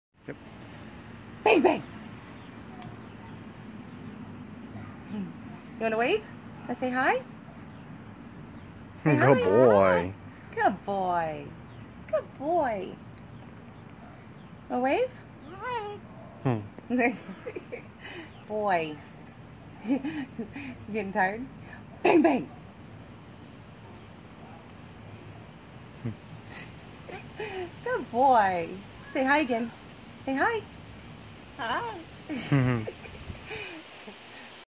Macaw playing dead
Blue and gold macaw Achilles playing dead, waving and saying Hi, you can follow my birds on instagram
Its brand new so more videos and images are coming